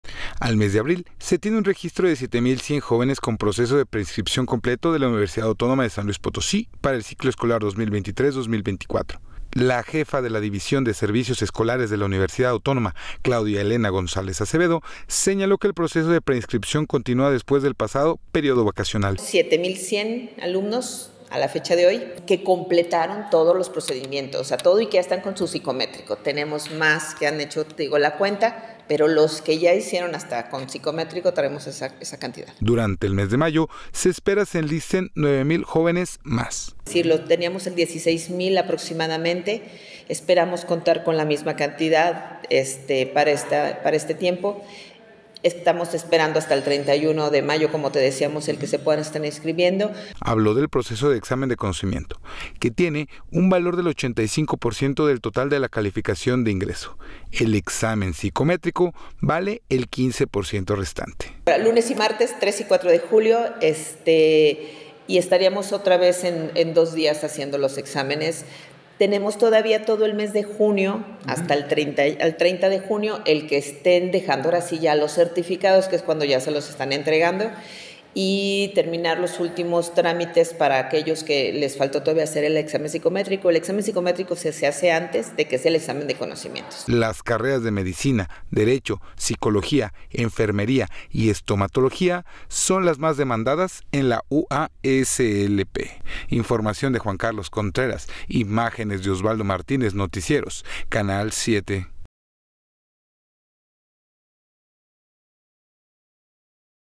Más de siete mil aspirantes se han preinscrito para el examen de admisión de la UASLP
SLP.- Al mes de abril se tiene un registro de 7 mil 100 jóvenes con proceso de preinscripción completo de la Universidad Autónoma de San Luis Potosí para el ciclo escolar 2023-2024.

La jefa de la División de Servicios Escolares de la Universidad Autónoma, Claudia Elena González Acevedo, señaló que el proceso de preinscripción continúa después del pasado periodo vacacional.

“Tenemos 7 mil 100 alumnos a la fecha de hoy (…) que completaron todos los procedimientos que ya están con sus psicométricos, tenemos más, pero es que ya hicieron hasta el psicométricos tenemos esa cantidad”.

Durante el mes de mayo se espera se enlisten nueve mil jóvenes más.

“Teníamos el 16 mil aproximadamente, esperamos contar con la misma cantidad para este tiempo, estamos esperando hasta el 31 de mayo como te decíamos, el que se pueden estar escribiendo”.

Habló del proceso de examen de conocimiento que tiene un valor del 85 por ciento del total de la calificación de ingreso, el psicométrico vale el 15 por ciento.

“Lunes y martes, tres y cuatro de julio y estaríamos otra vez en dos días haciendo los exámenes; tenemos todavía todo el mes de junio hasta el 30 de junio el que esté dejando ahora sí ya los certificados que es cuando ya se lo están entregando, y terminar los últimos trámites para que para aquellos que les faltó hacer el examen psicométrico, el examen psicométricos que sea antes que el examen de conocimientos”.

Las carreras de medicina, derecho, psicología, enfermería, estomatología son las más demandadas en la UASLP.